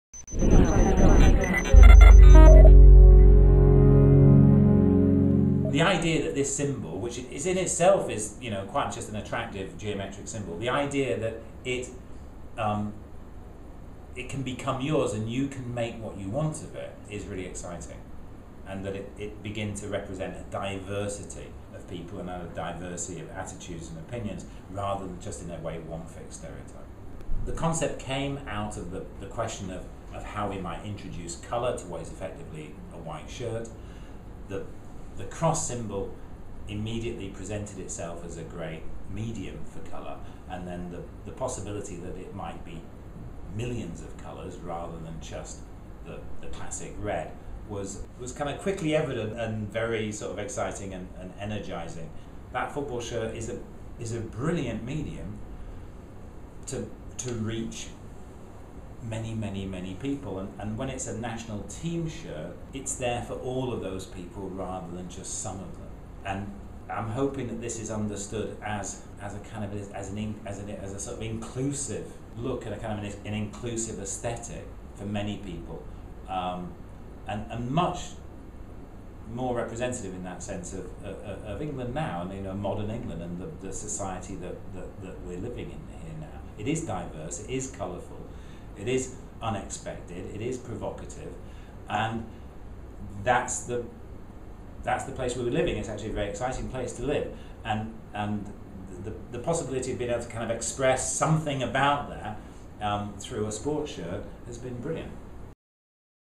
Peter Saville.mov
Peter Saville talks us through his design for the new England football shirt.